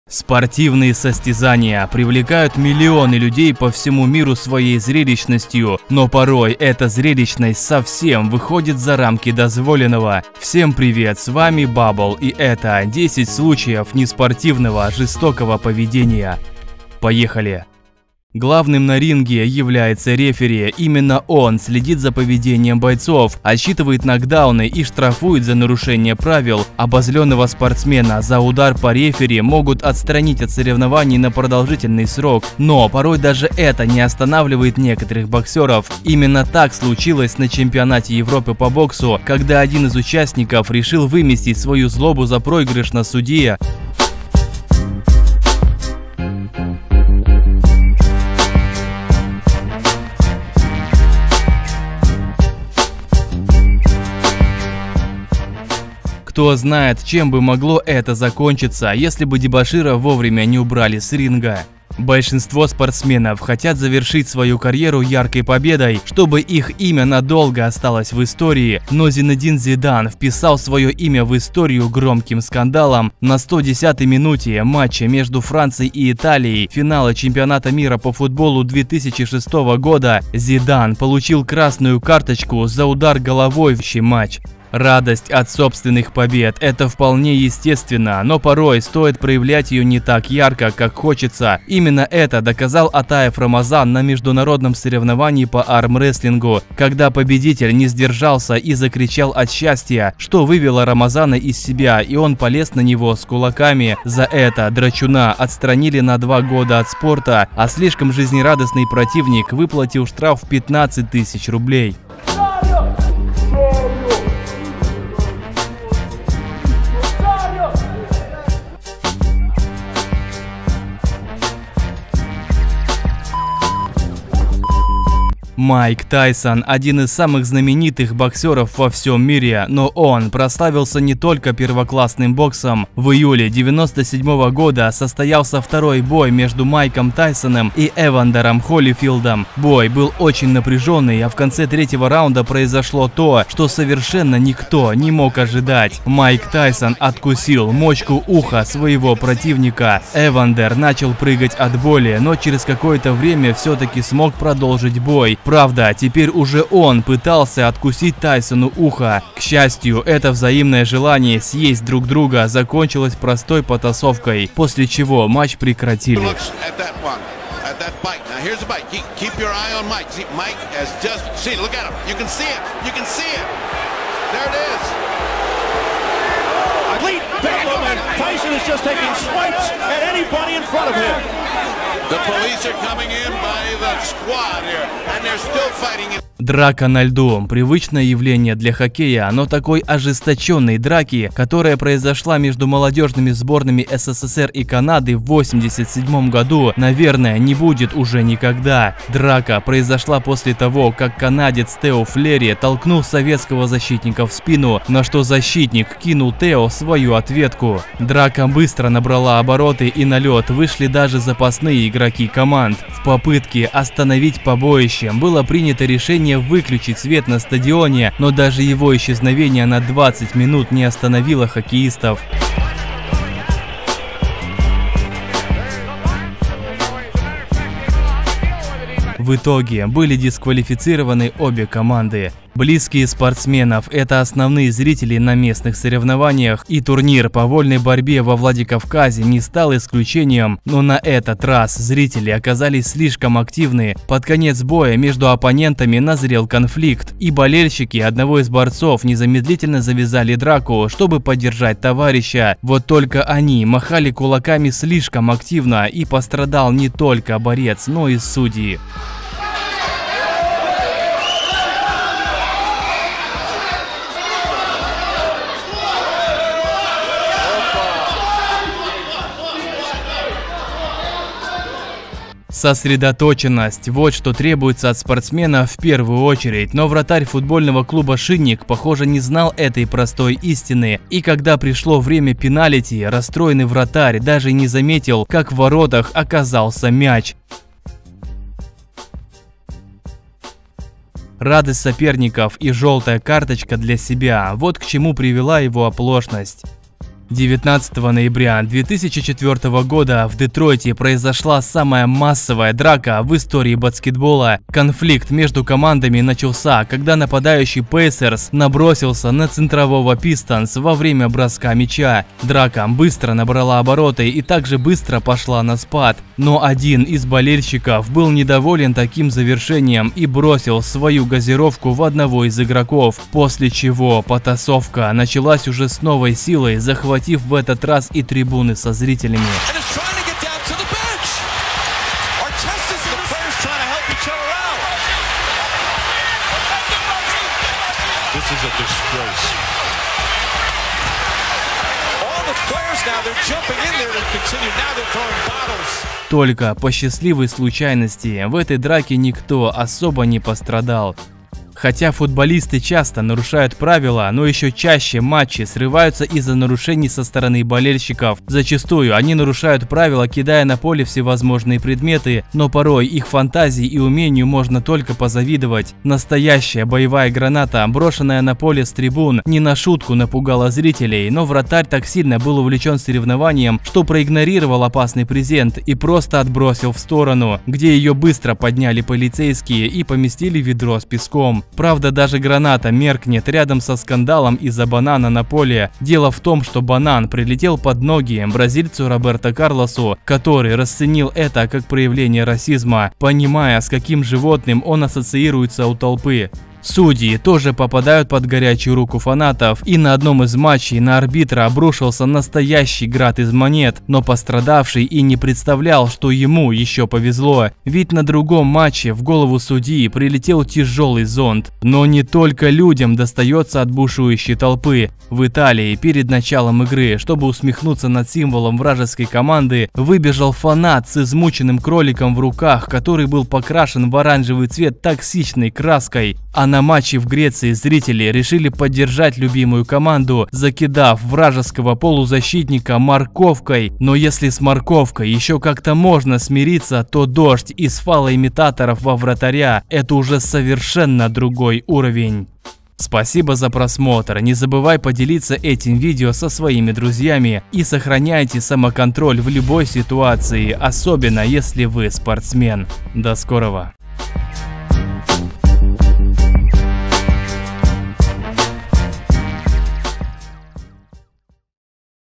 10 случаев неспортивного жестокого поведения
Десять случаев явного неспортивного поведения. Уверен, многие из них вы просто не видели, но Тайсона вспомнят многие, это просто жесть!

Спорт и здоровье всегда были синонимами, но о каком здоровье может идти речь в нашей сегодняшней подборки жести во время спорта. Разве что для хоккеистов это нормально. А вообще, многие случаи просто выходки неадекватов.